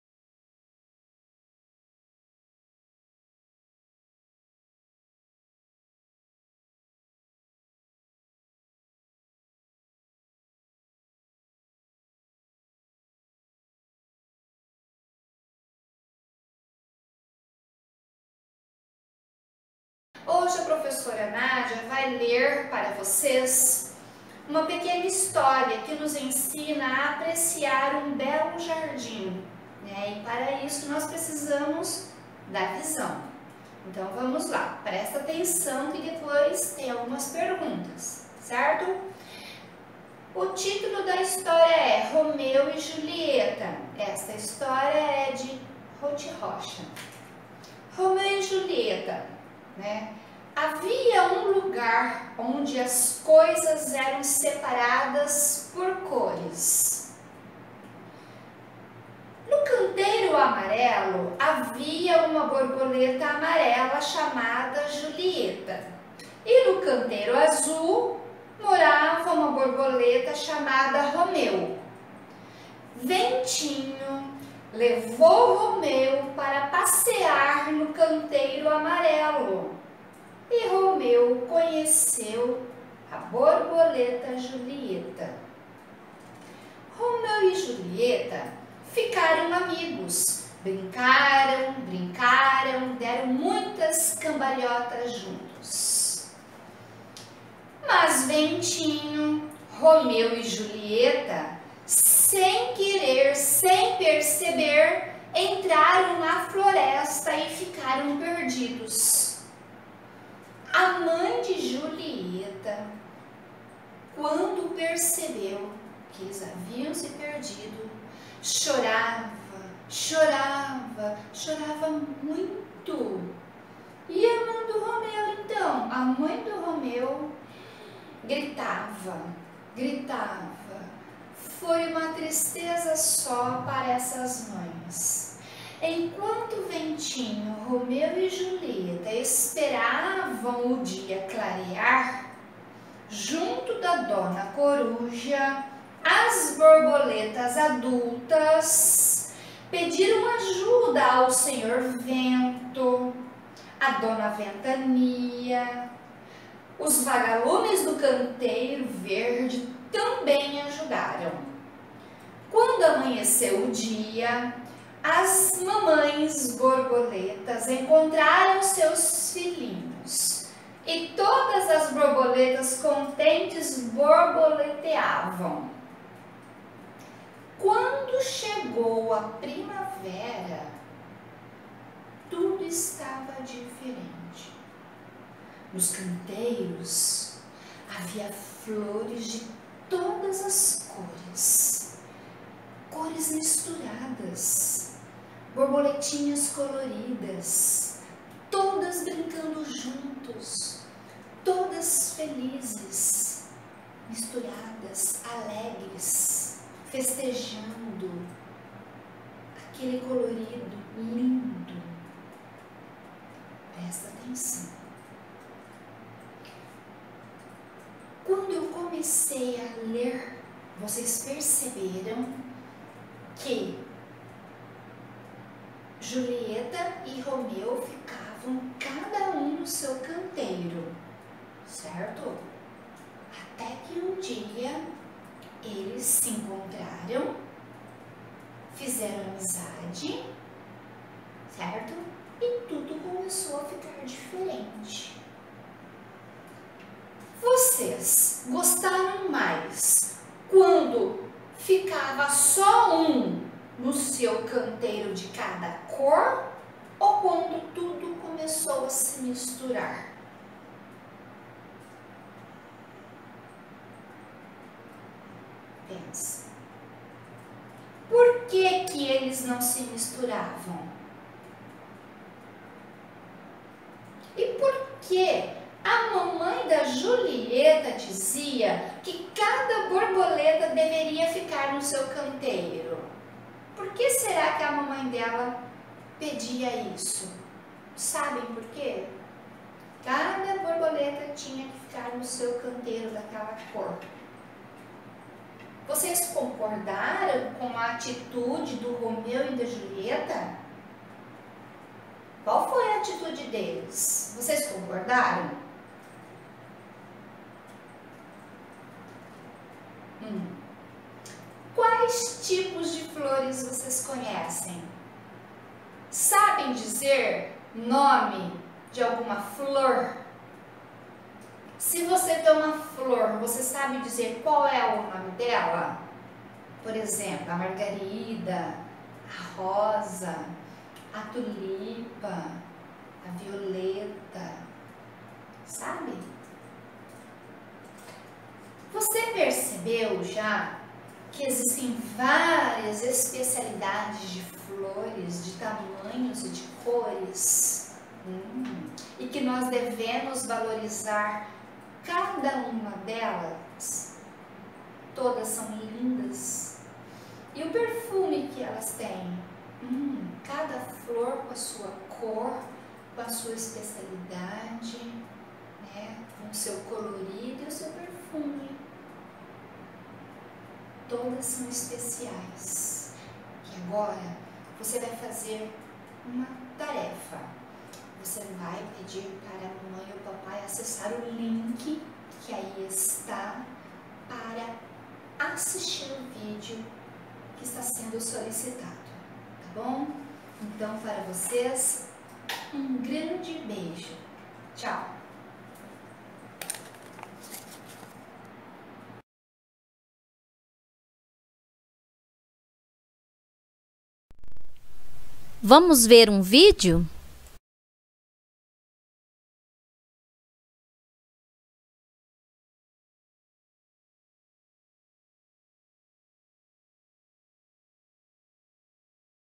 Pre1 Aula004 NatSoc 12Maio
Assunto da Aula: Romeu e Julieta - uma história de amizade e convivência. Produção: Professora Nádia Roncada.